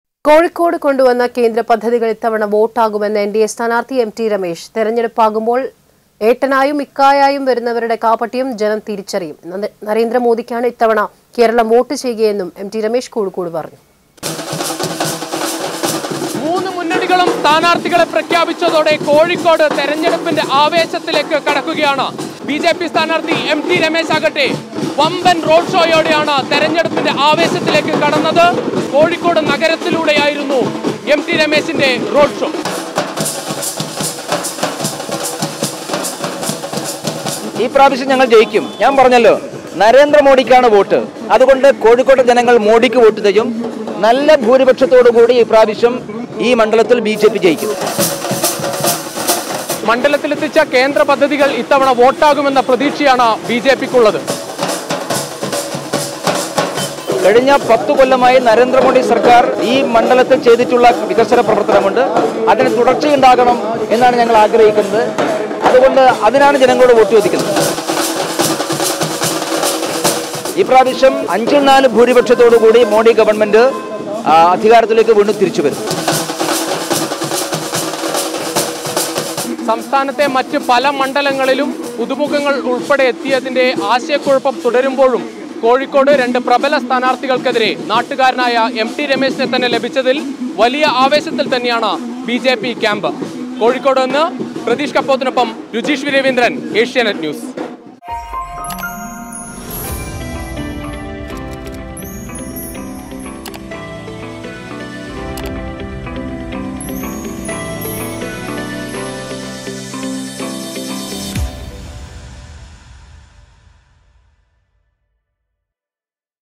'ഇത്തവണ വോട്ട് മോദിക്ക്, വൻ ഭൂരിപക്ഷത്തോടെ ബിജെപി വിജയിക്കും'
ഇത്തവണ വോട്ട് മോദിക്ക്, വൻ ഭൂരിപക്ഷത്തോടെ ബിജെപി വിജയിക്കുമെന്നും എംടി രമേശ്; ആവേശത്തോടെ പ്രവർത്തകർ